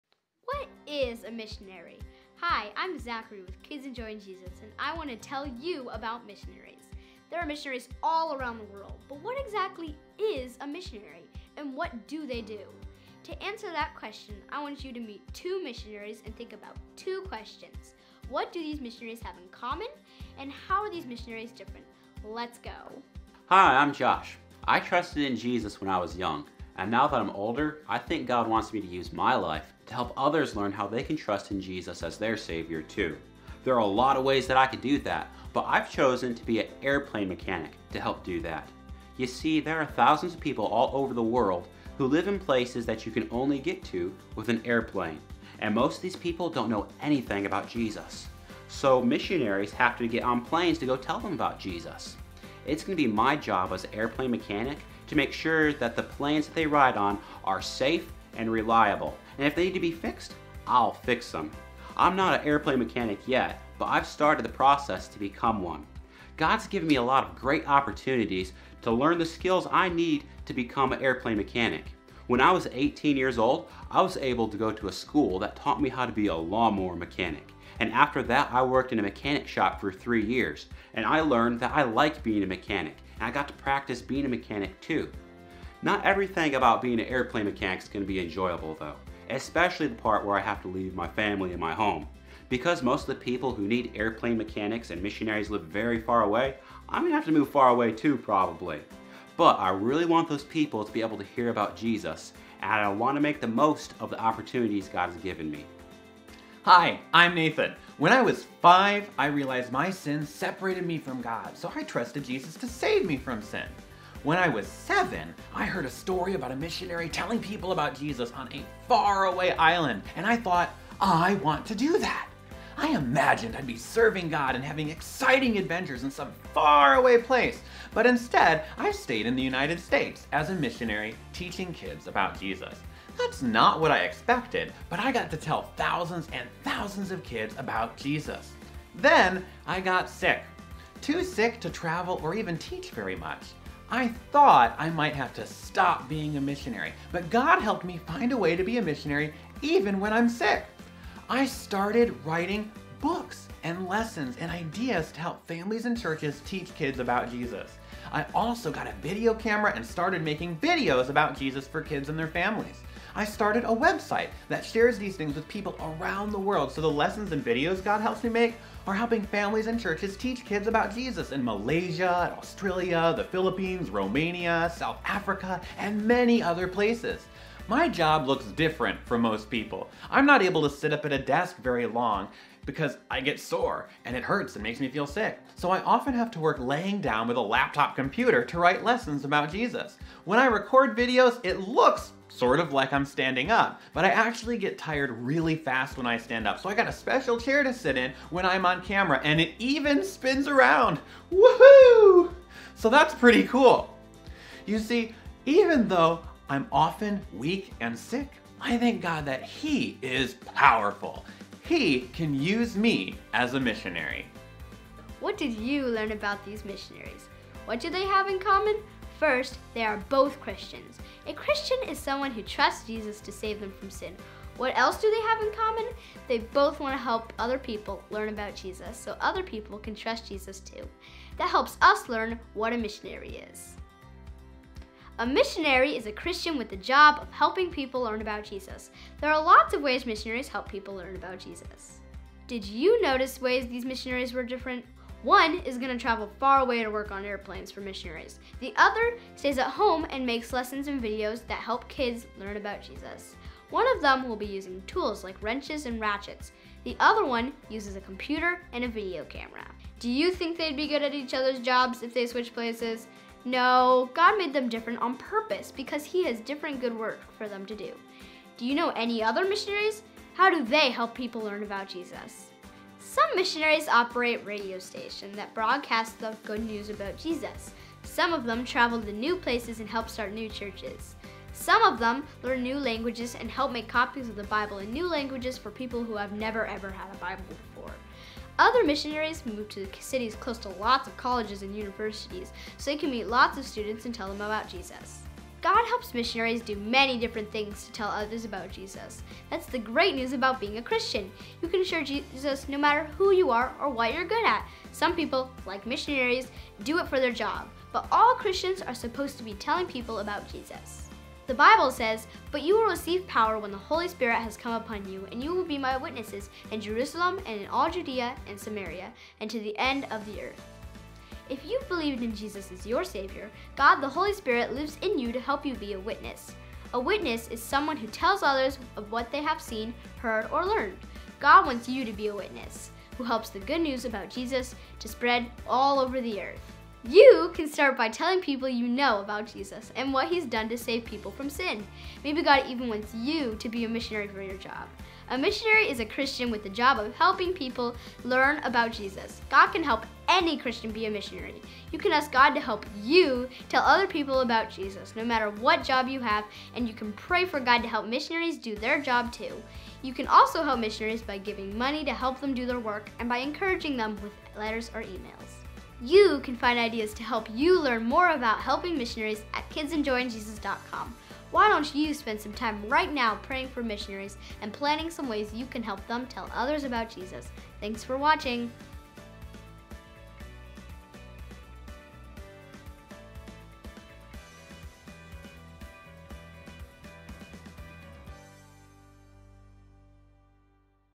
What is A Missionary? | Lesson for Kids
What is a missionary? In this video lesson, kids will learn that a missionary is a Christian with the job of helping people learn about Jesus. Kids will enjoy meeting some missionaries and discovering many different ways God uses missionaries to spread the Good News about Jesus all around the world. This lesson helps kids learn they can be a witness too by telling others about Jesus and helping missionaries share the Gospel.